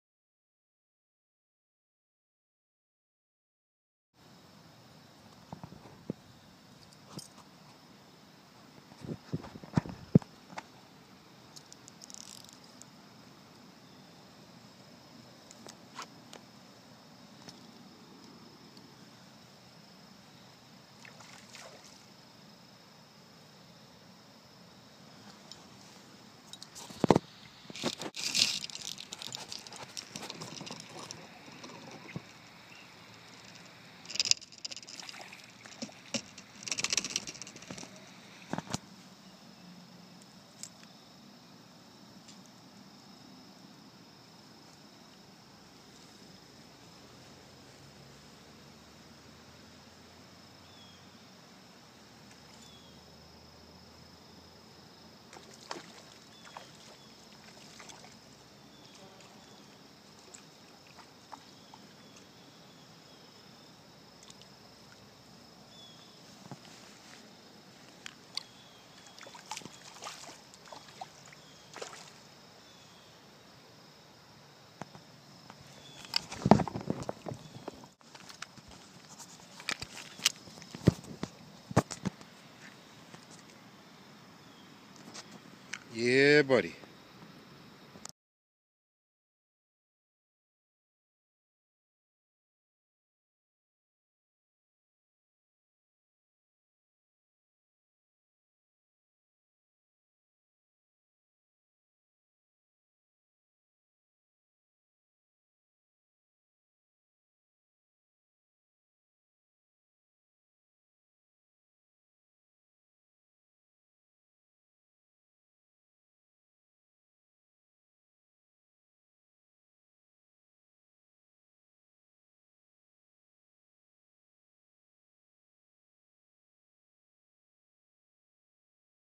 Flyfishing East Branch NY
9-22-12 Fly Fishing 8am-10am Connected with this Beauty Brown Trout
it has the whole story and more